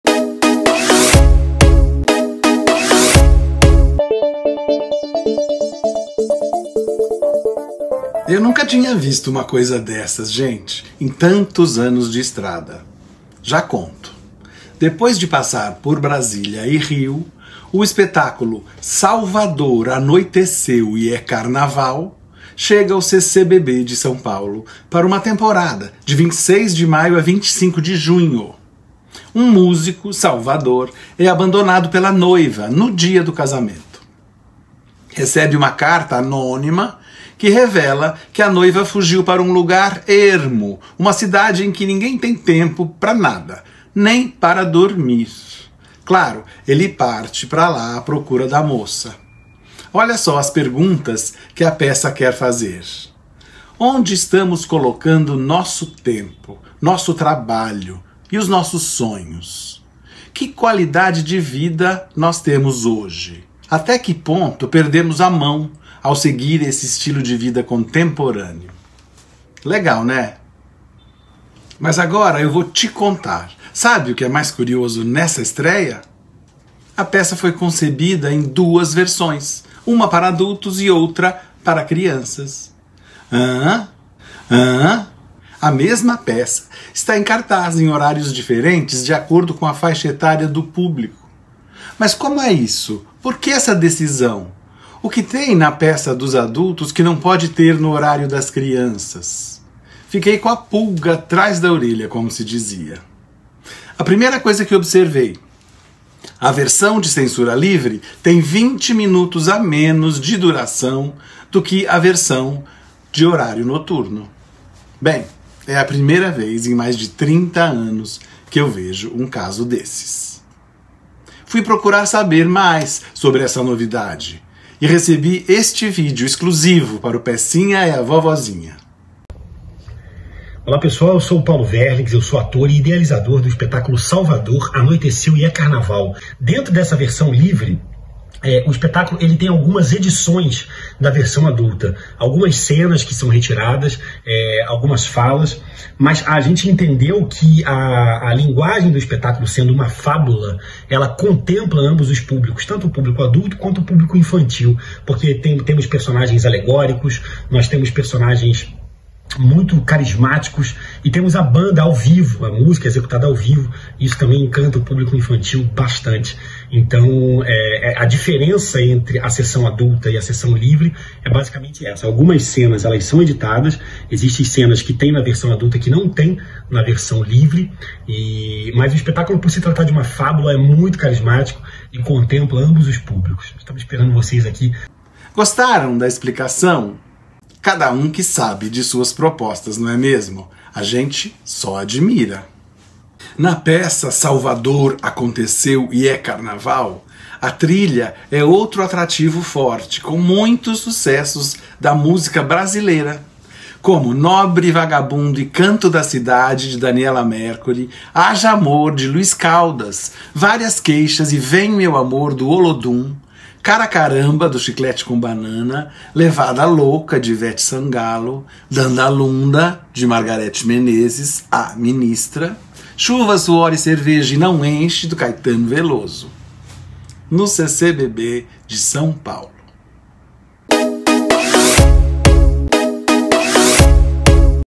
Dois espetáculos em um só espetáculo. E se essa moda pegar?
Salvador, Anoiteceu e é Carnaval.
Com dramaturgia de Marcéli Torquato e direção de Vilma Melo, a peça foi idealizada pelo ator Paulo Verlings.
Fotos: Paula Kossatz/Divulgação
Local: Centro Cultural Banco do Brasil São Paulo
Temporada: 26 de maio a 25 de junho de 2023.
Horários:
Versão +14 - Sextas, às 19h; sábados e domingos, às 17h
Versão Livre - Sábados e domingos, às 11h
Classificação indicativa: Livre
Ingressos: R$ 30 (inteira) e R$ 15 (meia-entrada), disponíveis e bilheteria do CCBB
Duração: 90 minutos (14 anos)
70 minutos (Livre)
Classificação: as sessões noturnas/vespertinas são recomendadas para maiores de 14 anos, já as matutinas são livres para todas as idades
Capacidade: 120 lugares
Endereço: Rua Álvares Penteado, 112 – Centro Histórico, São Paulo – SP

Pecinha É a Vovozinha.
Jornalista responsável e apresentador: DIB CARNEIRO NETO.